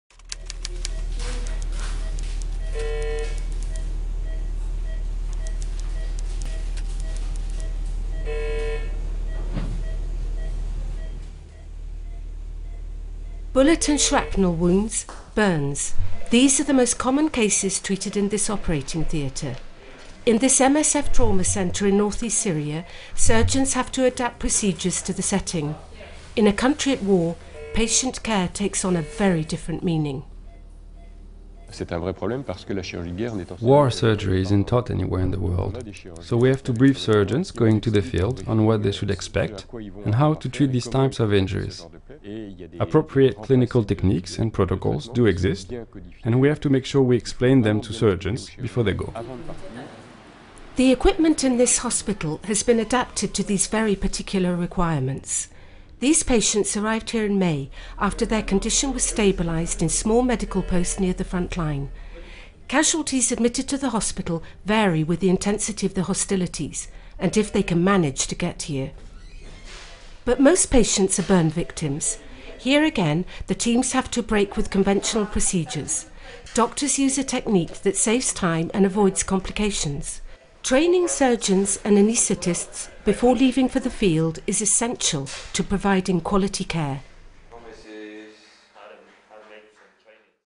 A Trauma Center in Northeast Syria
In a Doctors Without Borders/Médecins Sans Frontières (MSF) trauma center in northeast Syria, surgeons must adapt procedures to the complex and challenging setting. In a country at war, patient care takes on a very different meaning.